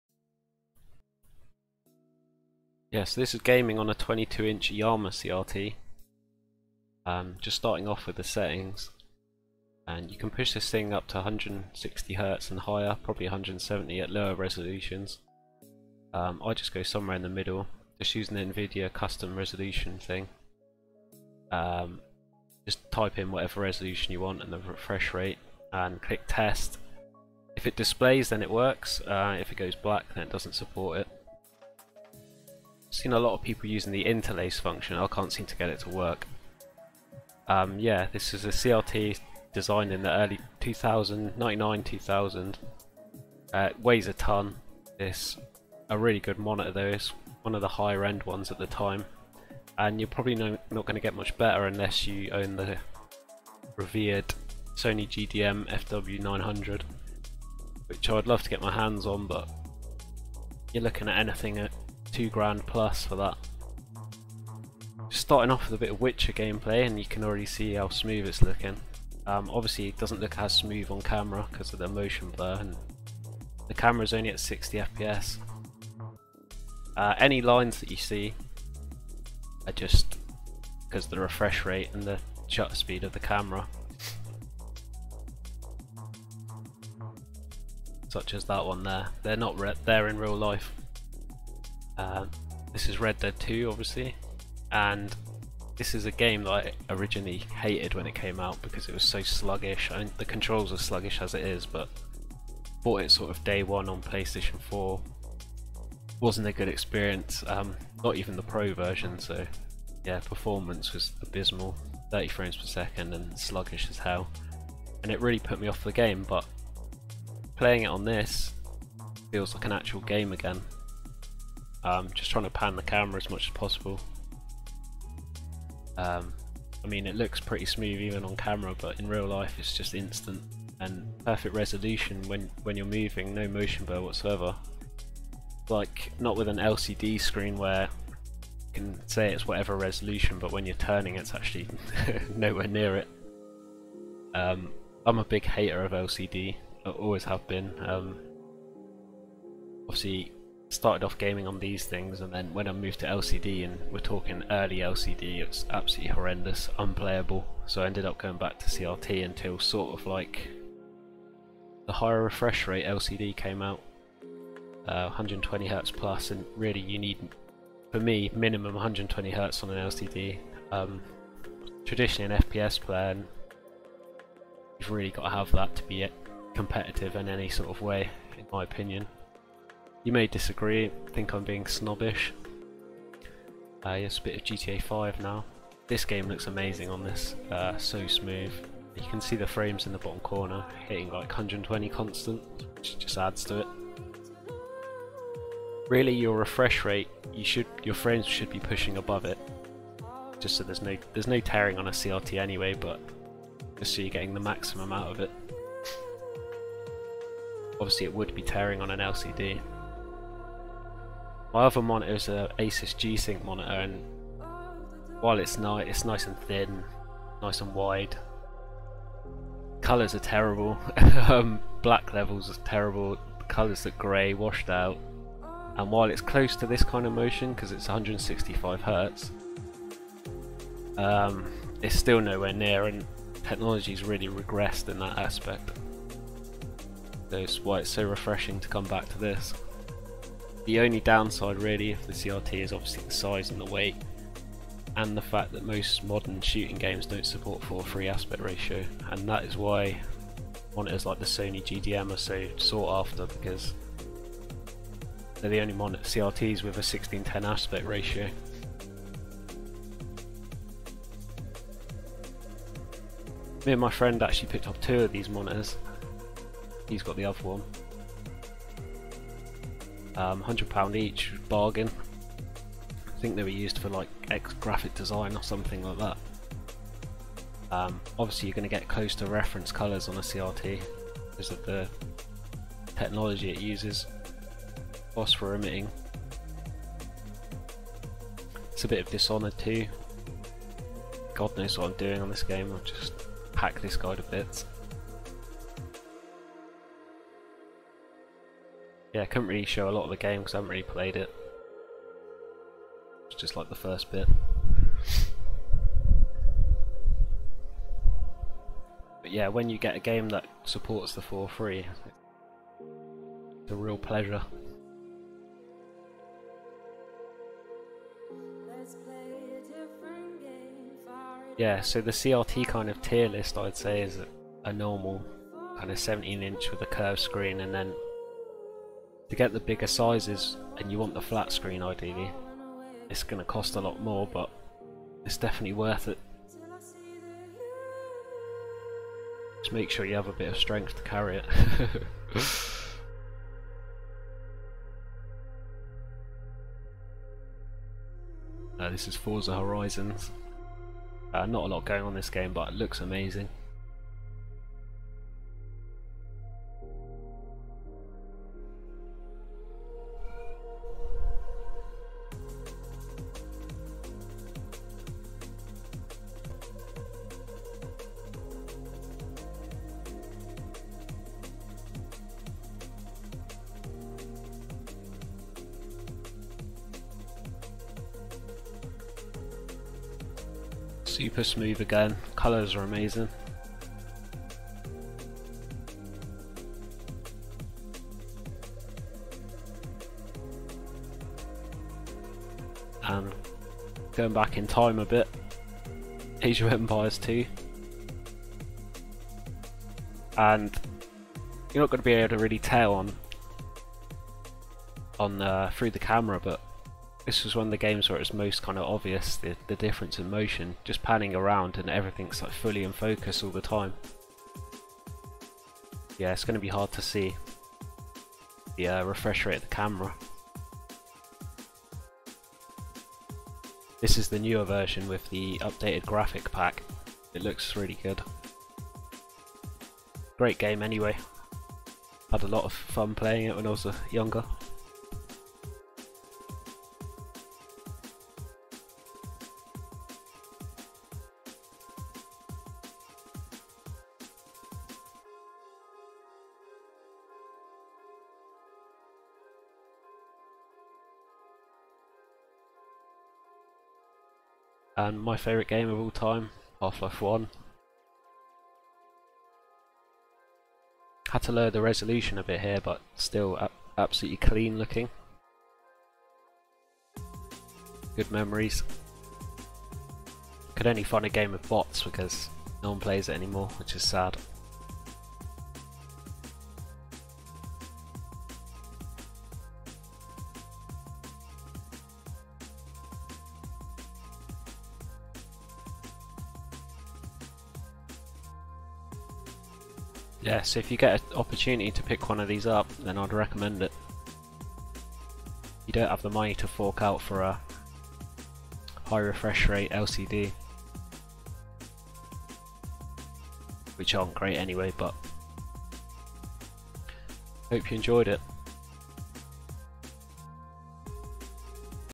Gaming on an Iiyama Vision Master Pro 510 22" Flatscreen CRT Monitor 120hz (2023)
A variety of games played on a recently obtained flatscreen CRT. The motion, colour and image quality put most modern LCD monitors to shame.

GPU: NVIDIA Titan XP
Adapter: Startech Displayport to VGA